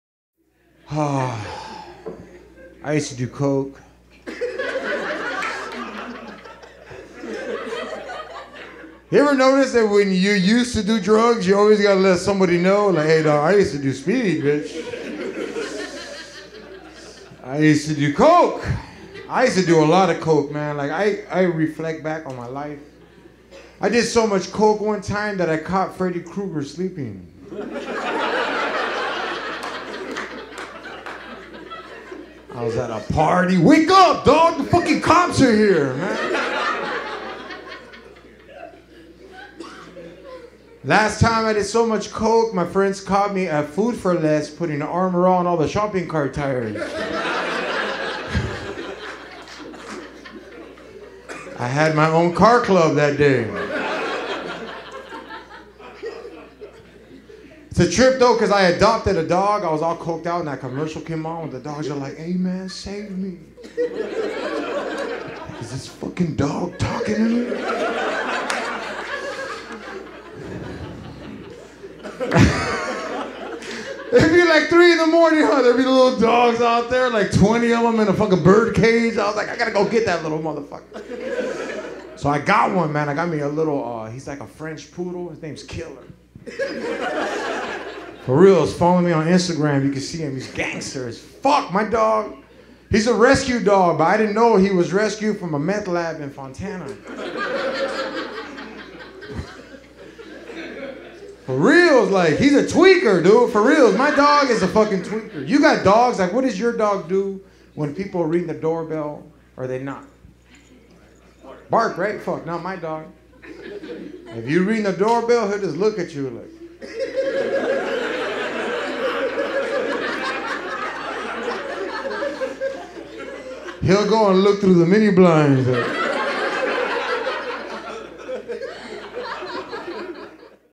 George Perez - All Coked Out (Stand Up Comedy)
George Perez is a special kind of inebriated.

For exclusive ad free content and longer videos, try out Laugh Factory VIP for 99 cents a month